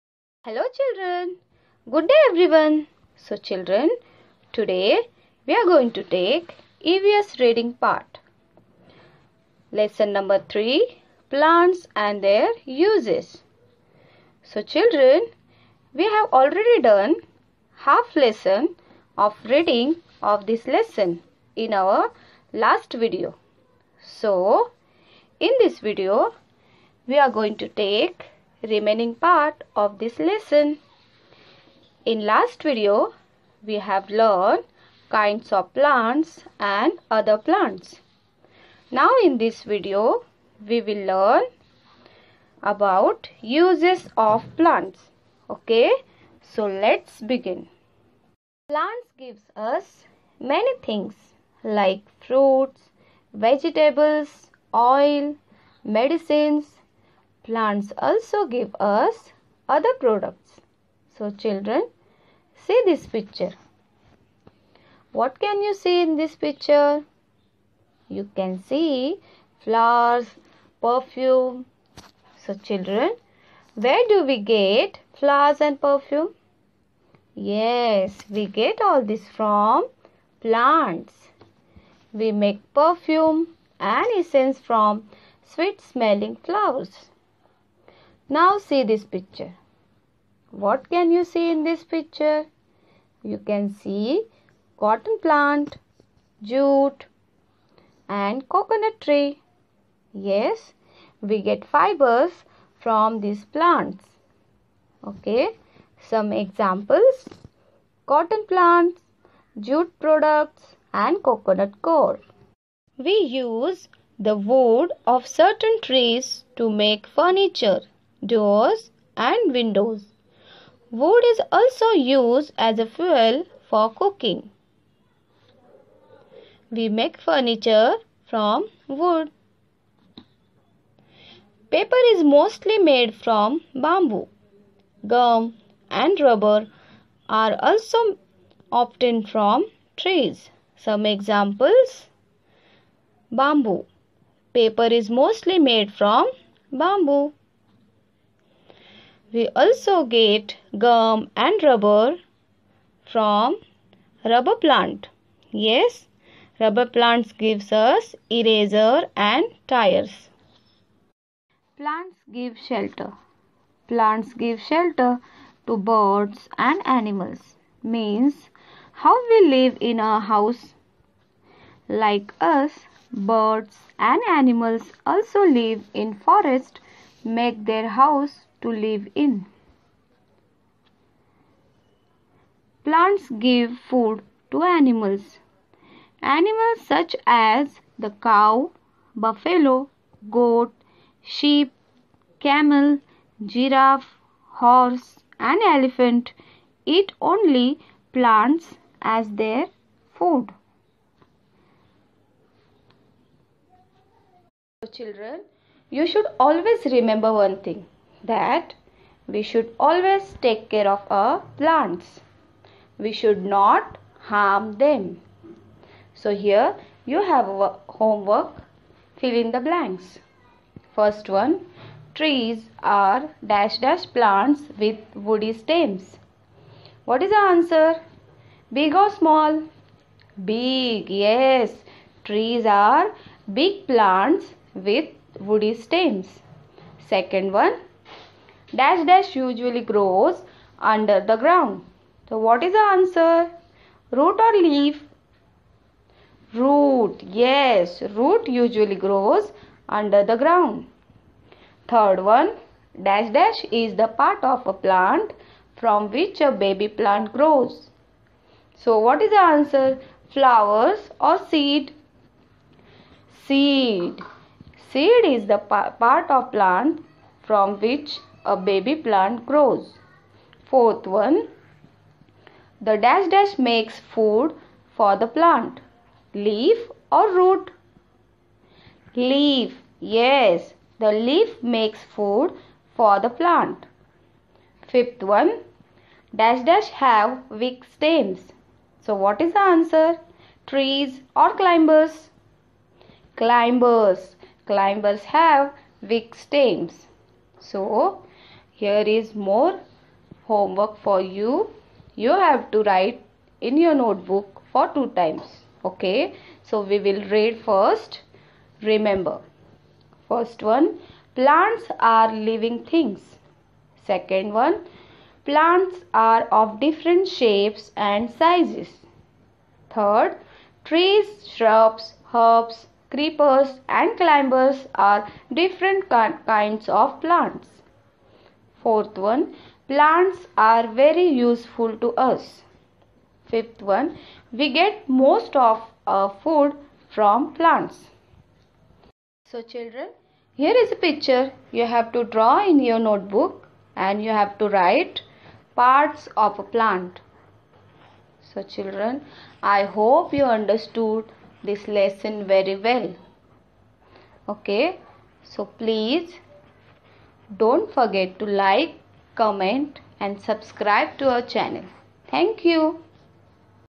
Std - ll, Sub -EVS, Lesson no - 3 , Plants and their uess , Pg no 15 to 16
1. Plants are living things .
2. Plants are of different shapes and sizes .
3. Trees, shrubs, herbs, creeper and climbers are different kinds of plants .
4. Plants are very useful to us .
5. We get most of our food from plants .